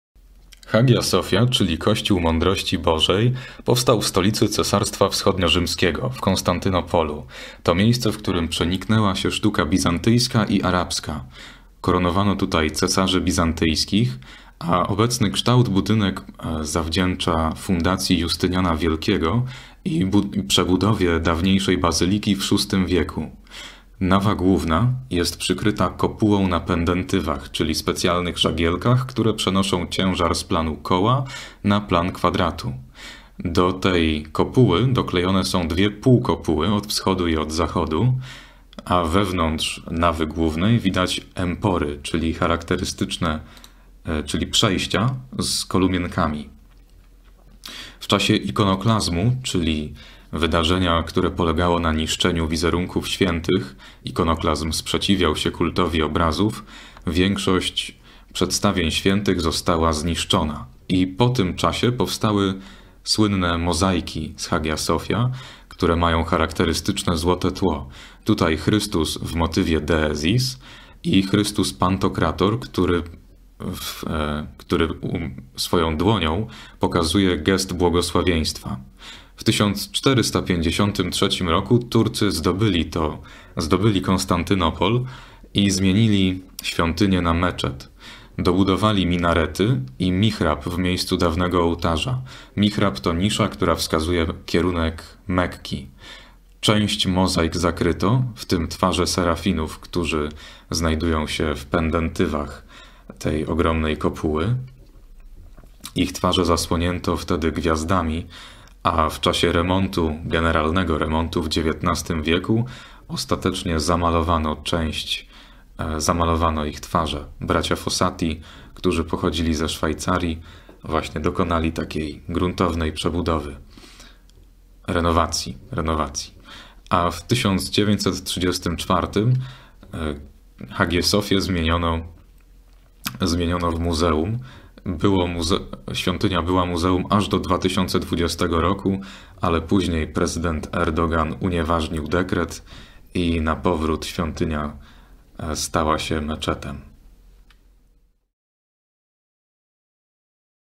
Hagia Sophia
Kościół Mądrości Bożej

Stolica Cesarstwa wschodnio-rzymskiego
Konstantynopol

sztuka bizantyjska i arabska

koronowano cesarzy bizantyńskich

obecny kształt
VI wiek
Justynian Wielki (na mozaice z San Vitale w Rawennie)

nawa główna
kopuła na pendentywach
żagielki
przenoszą ciężar z planu koła na kwadrat

półkopuły
wschód zachód

empory
przejścia

Ikonoklazm
niszczono wizerunki świętych
sprzeciwiał się kultowi obrazów

mozaiki
złote tło

Chrystus motyw Deesis

Chrystus Pantokrator
dłoń
gest błogosławieństwa

1453 Turcy zdobycie
zamiana na meczet

minarety
mihrab w miejscu ołtarza
nisza wskazuje Mekkę

część mozaik zakryto
twarze serafinów
pendentywy
gwiazdami

Remont w XIX wieku
bracia Fossati

1934 do 2020 muzeum
2020 meczet